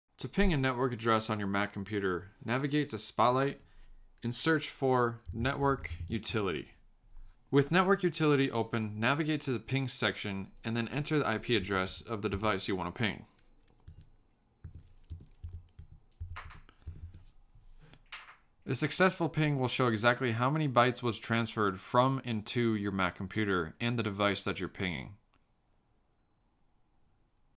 How to Ping a Network Address on a Mac
This video will show you how to ping a network device or address using a Mac.